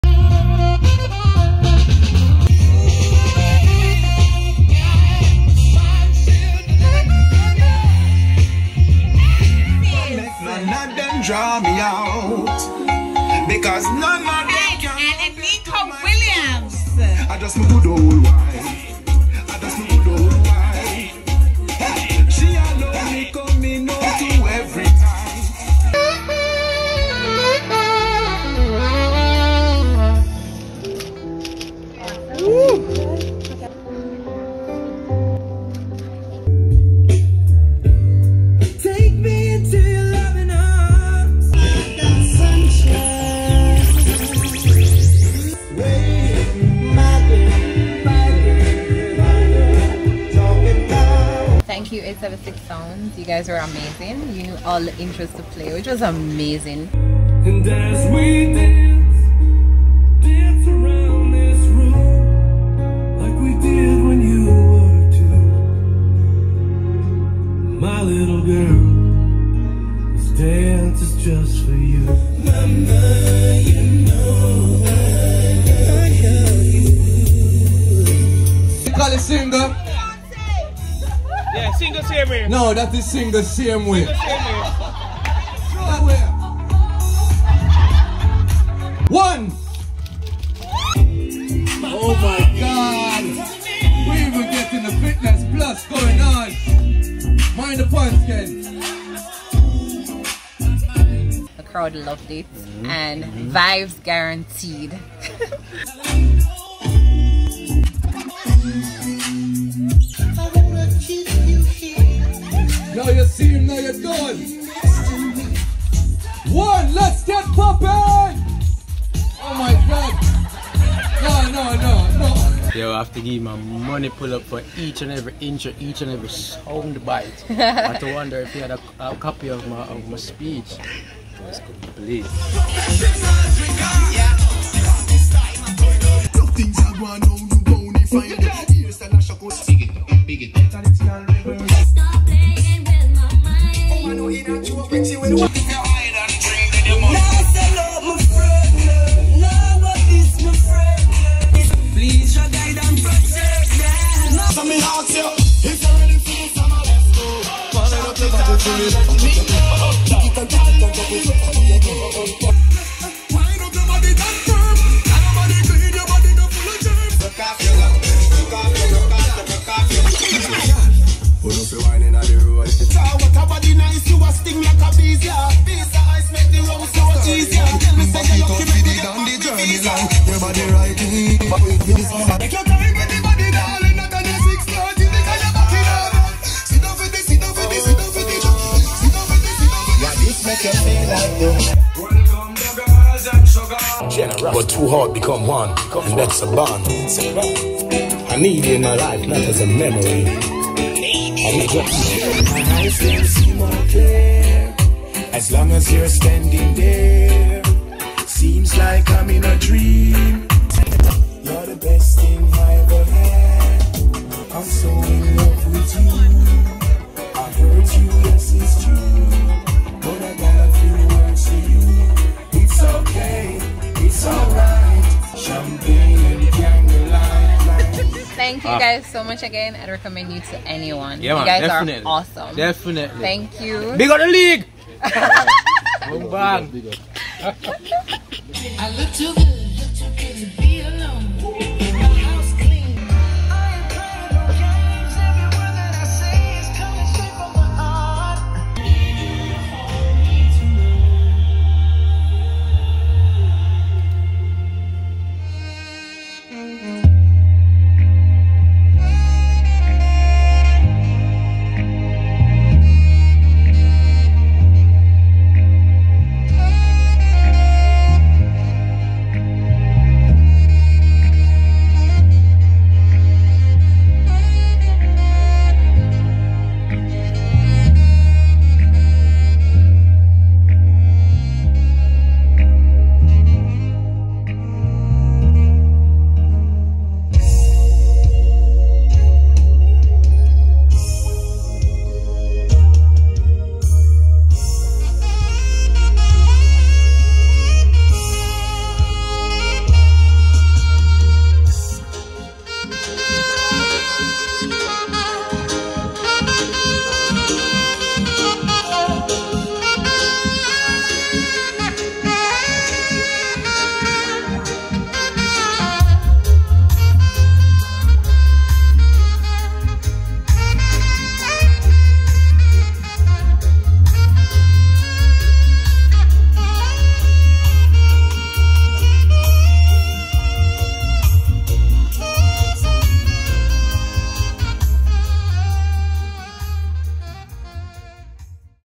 CraigBank Estate Pandemic Wedding - Anneika & Kent Williams
tied the knot in the Hills of St. Andrew. With so much love in the air, their micro wedding at the CraigBank Estate was nothing short of spectacular.

Planning / Decor : Petals and Promises
officiant: Dr. Astor Carlyle
Photo / Livestream: Dash Productions
Catering: Eskay Caterers
MC: Walter Milwood
Saxophonist: Verlando Small
Cake: Jodi-Ann Paul @jodicakes_
MUA: Sue Gregg

We hope you enjoyed this highlight of Ket & Anneika.   Cheers!